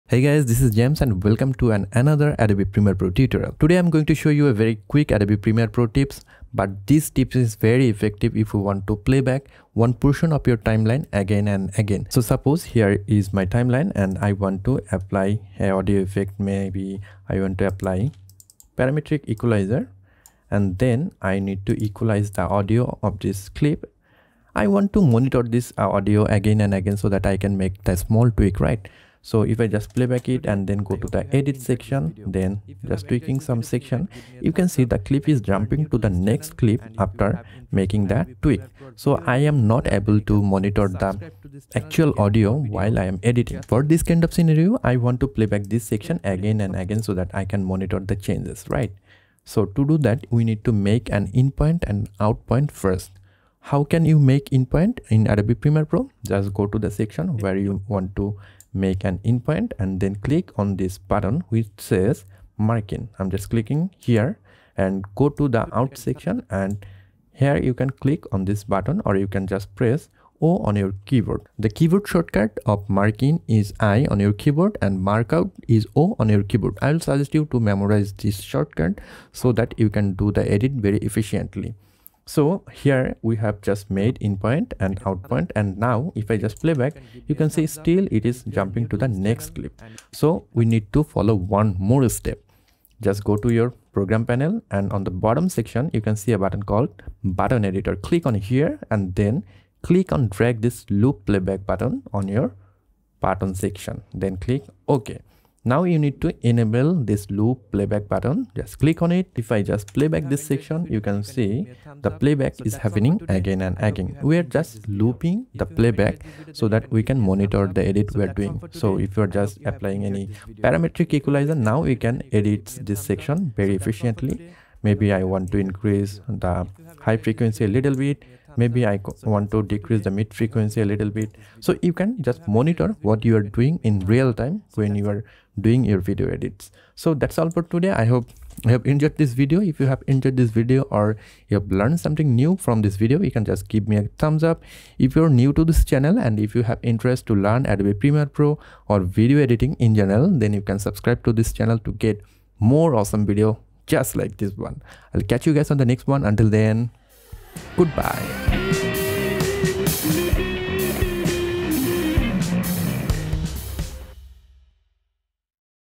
How to Loop Playback in Premiere Pro
In Premiere Pro when you are doing video editing and making changes to certain parts of your clip you may need to playback that section again an again. It is called loop playback. During today's tutorial, I'll show you how you can enable loop playback in Premiere Pro.